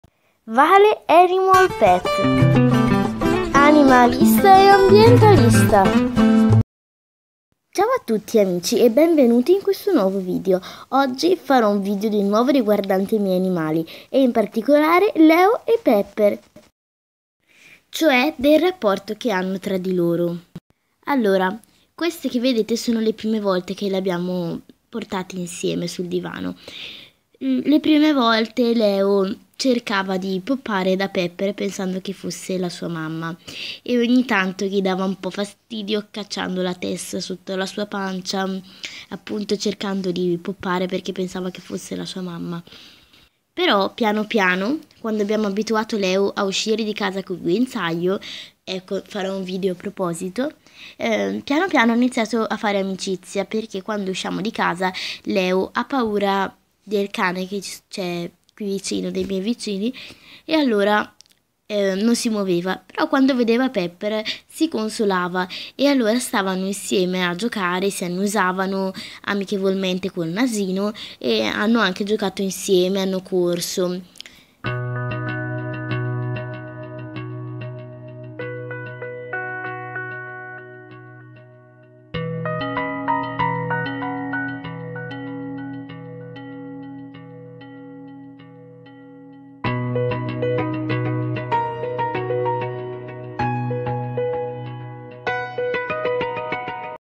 CONIGLIO e GATTO AMICI?! | è possibile?- PEPPER E LEO
Il mio Pepper è il mio Leo sono AMICI!!!
SCOPRI COME!😊💖🐱🐰🐱🐰🐱🐰

CIAO!